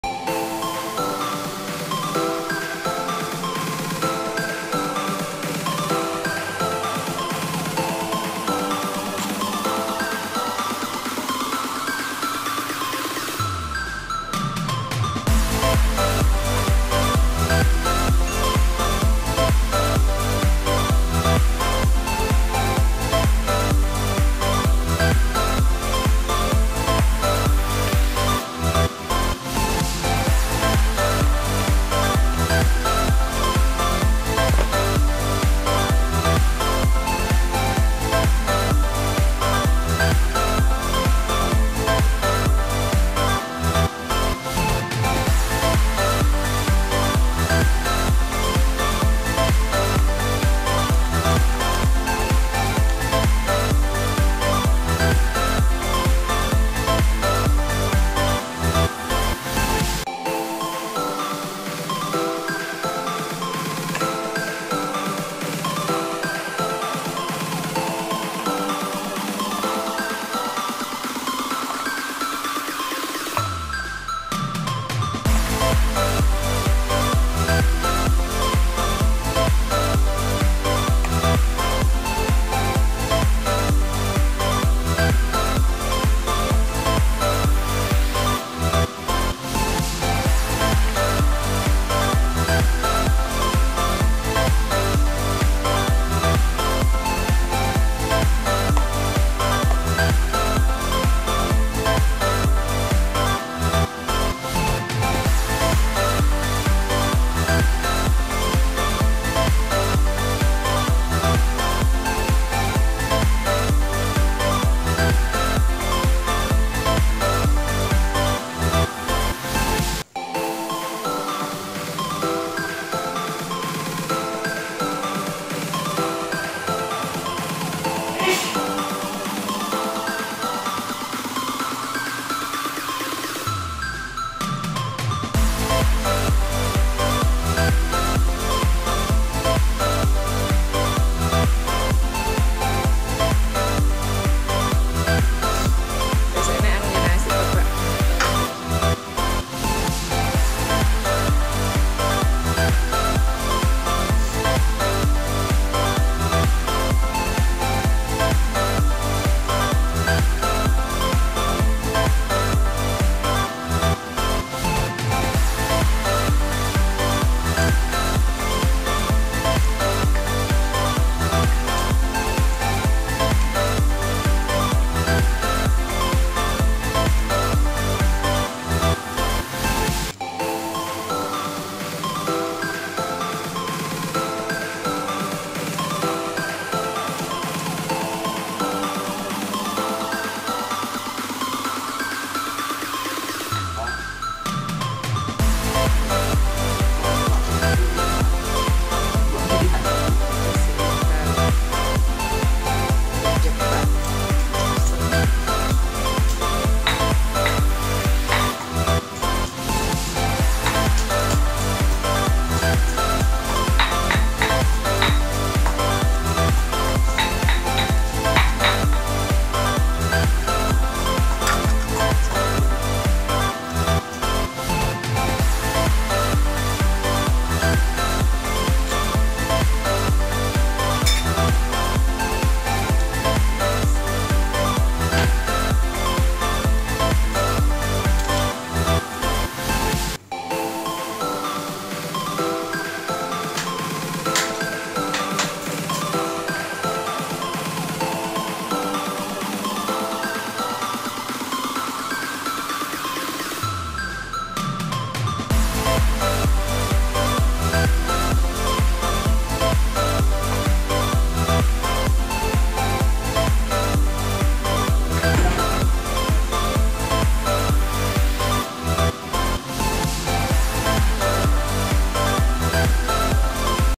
Resep perkadel pakis II Rasanya enak dan gurih, cara bikinnya mudah banget
Menu sederhana resep perkadel pakis, rasanya enak makananenak resepmakanan paling mudah # menu hari hari

Bahan
Sayur pakis 1 ikat
Tepung sajiku 150 gr
Air secukupnya
Minyak di pakai menggoreng